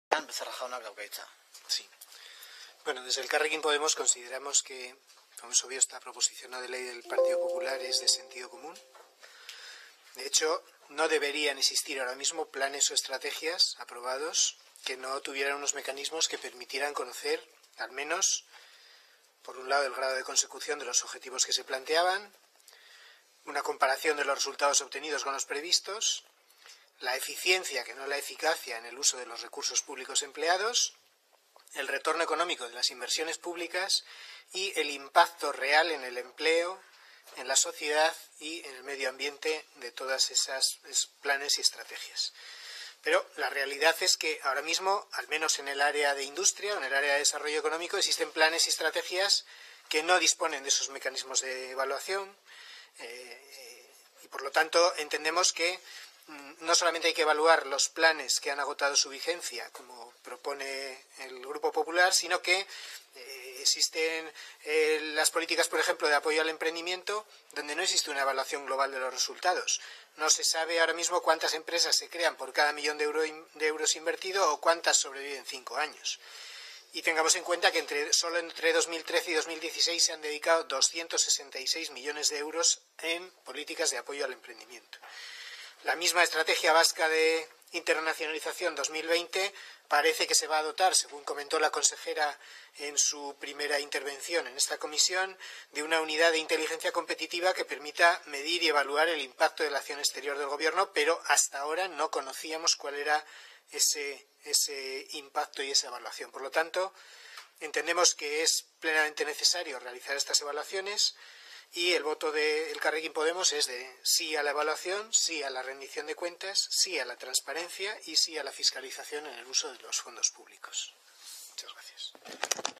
Rendición de cuentas, transparencia y fiscalización del uso de fondos públicos
Apoyamos en Parlamento vasco una iniciativa que solitiva informes de evaluación de los planes del área de Industria y de la estrategia Basque Country. Decimos sí a la  rendición de cuentas, transparencia y a la fiscalización del uso de fondos públicos. ASí lo ha argumentado el parlamentario verde José Ramón Becerra.